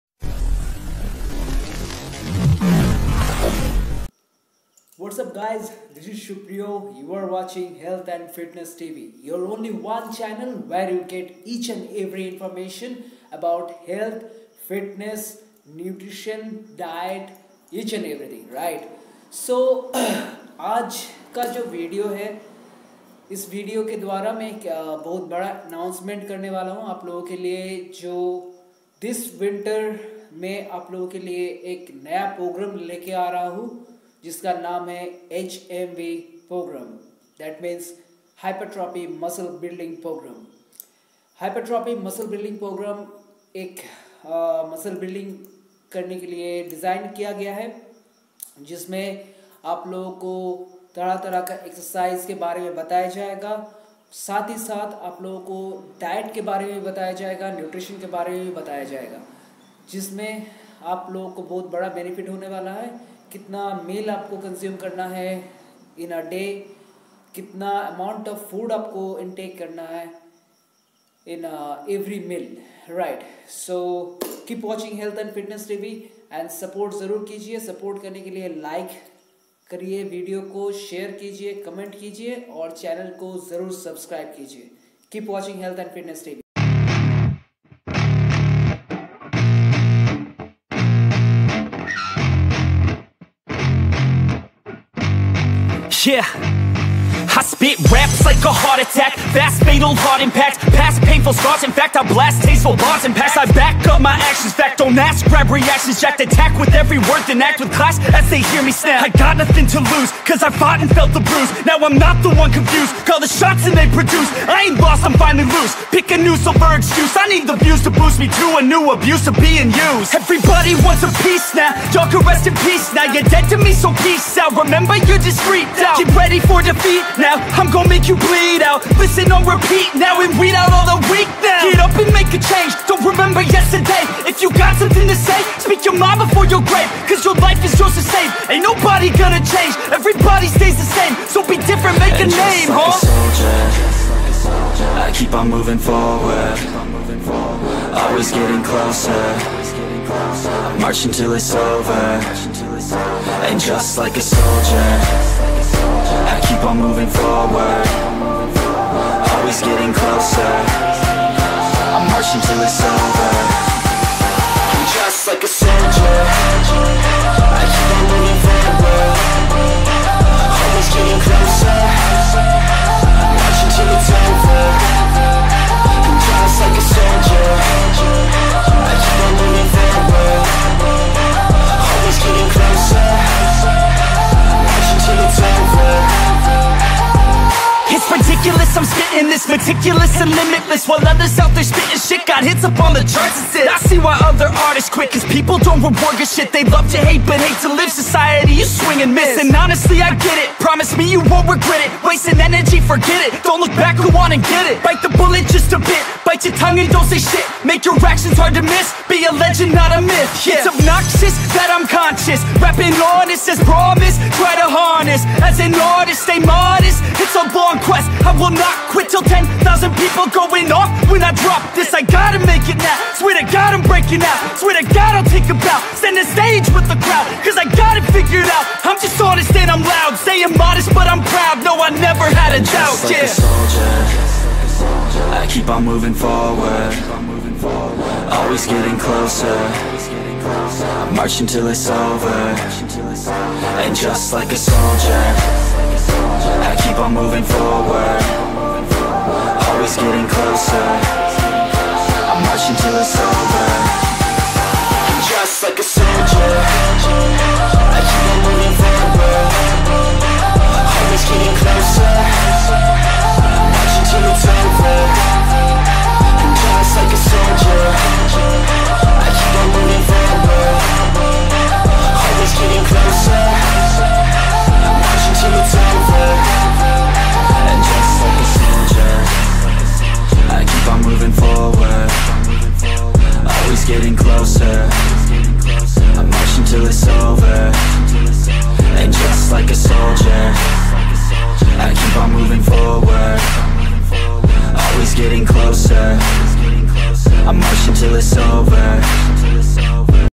Introducing Hypertrophy Muscle Building Program
Guys in this Winter we brought you a great series of Exercise program only for you. It is one & only HMB program.
Hypertrophy Muscle Building program duration will be 8 weeks.
It is the only one program through which you will get a great volume of muscle.
So, please watch the series of HMB which will be launched on 25th December 2019.
So keep watching Health and Fitness TV,  also